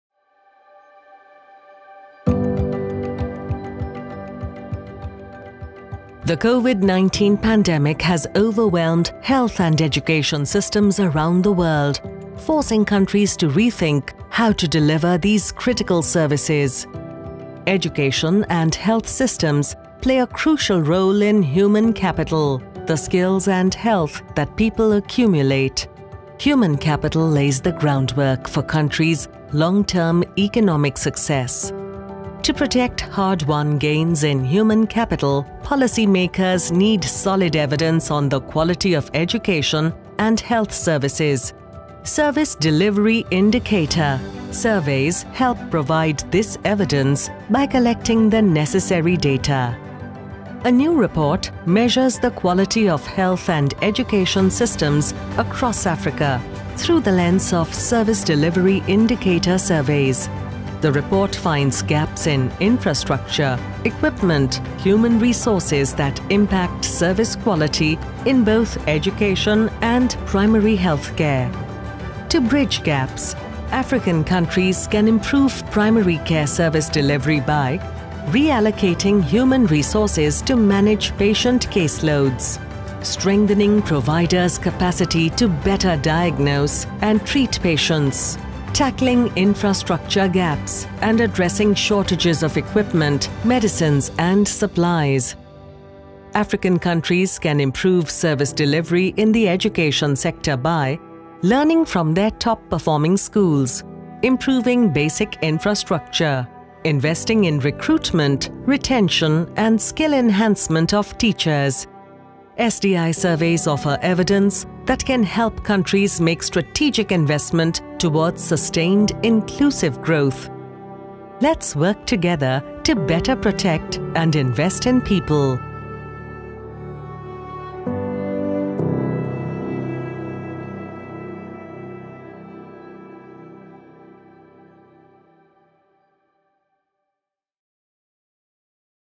Are Health and Education Systems Delivering for Citizens? Insights from Service Delivery Indicators
The speed with which the spread of COVID-19 pandemic has overwhelmed the health and education systems around the world forced countries to rethink and rapidly re-adjust the modes of delivery of these critical services. More widely implemented and better-quality measurement of the key factors that determine quality of service delivery in health care and education is now more critical than ever in helping countries emerge from the COVID-19 crisis having protected the hard-won gains in human capital. To this end, policy makers need sound evidence on the quality and accessibility of education and health services. Service Delivery Indicator (SDI) surveys help provide this evidence by collecting data at the facility level – in primary schools, clinics, and hospitals – across the public and private sector and in urban and rural areas.